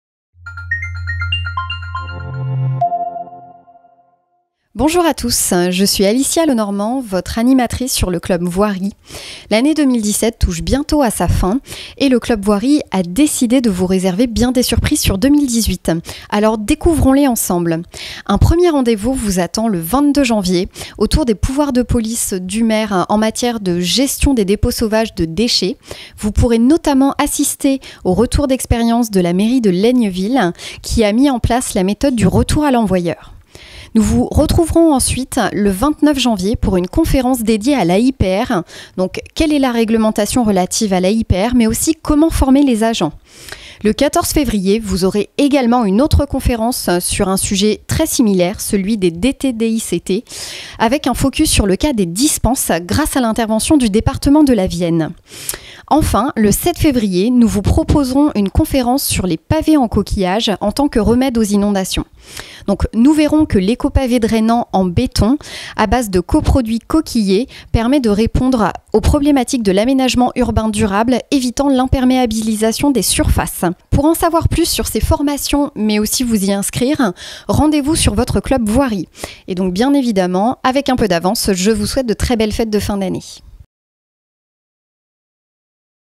Formations - Club Voirie - 1er trimestre 2018
Retrouvez les prochaines dates de formations ci-dessous :

- 22 janvier à 14h30 : Quel est le rôle du Maire en matière de gestion des dépôts sauvages de déchets ? - 00:18
- 29 janvier à 10h30 : AIPR: Réglementation et formation des agents - 00:32
- 14 février à 10h30 : DT-DICT : Les cas de dispenses - 00:43
- 7 février à 10h30 : Remédier aux inondations grâce aux pavés en coquillage - 00:55

Pour des informations et vous inscrire à la communauté Éducation, suivez le lien suivant :